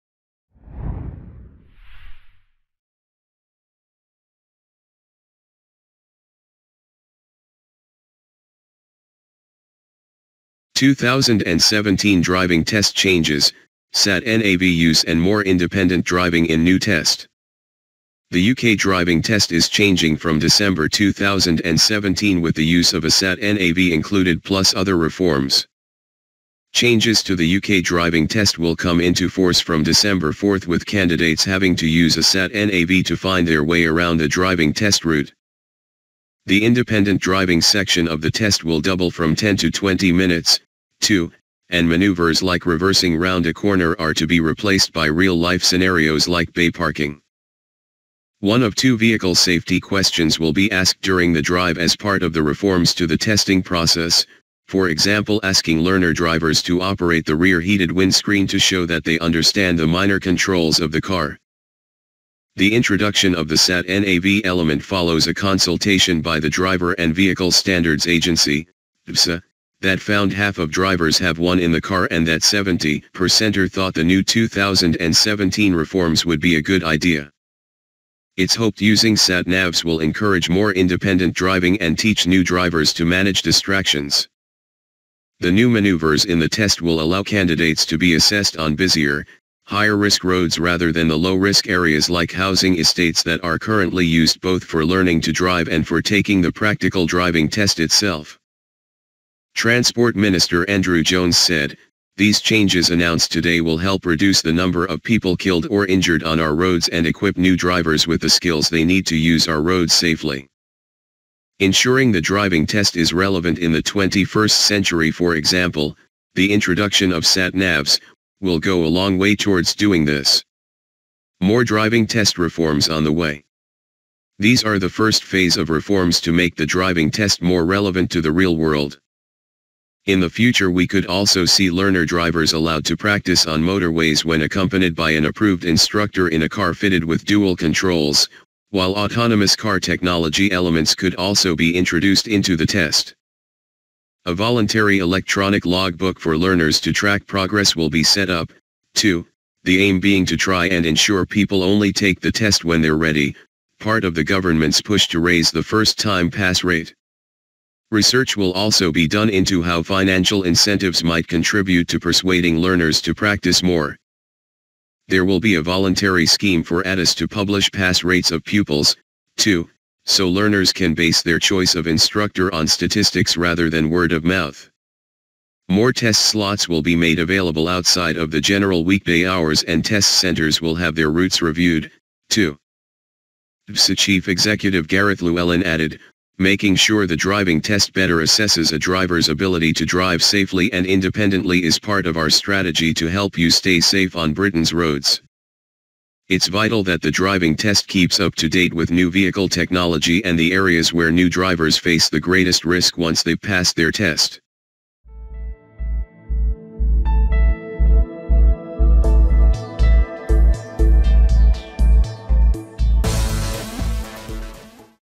Car Review - 2017 Driving Test Changes: Sat Nav Use And More Independent Driving In New Test
Read newspaper:
Source: autoexpress.co.uk